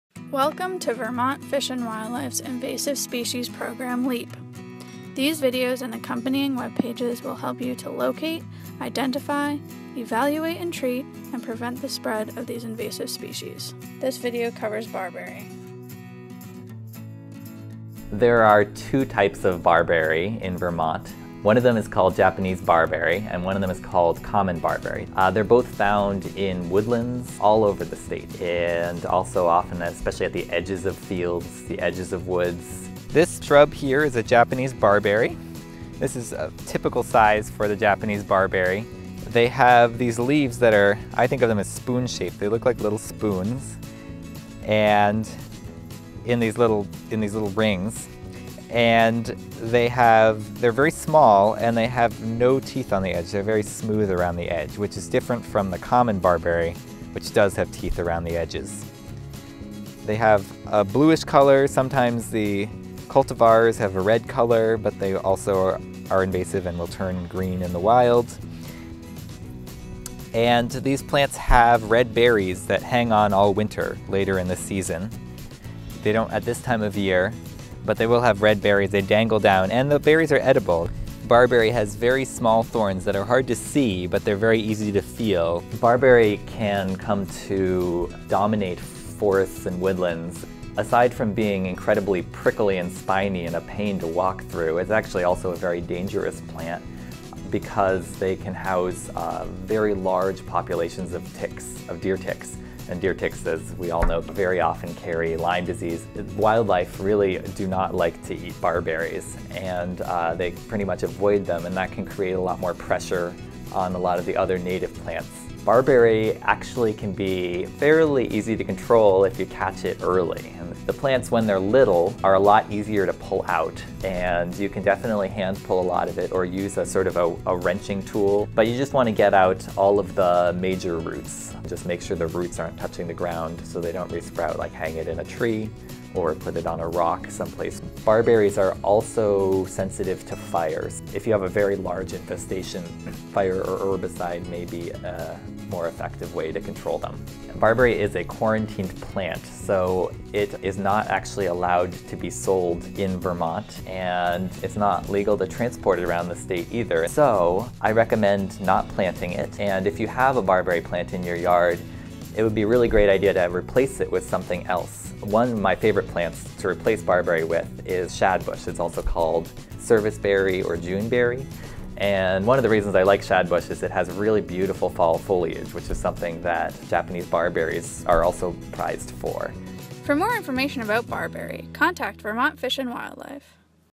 Barberry
LIEP into action for invasive species control! Learn how to Locate, Identify, Evaluate and treat, and Prevent the spread of Japanese and common barberry.

Created by ECO AmeriCorps member Marissa Ng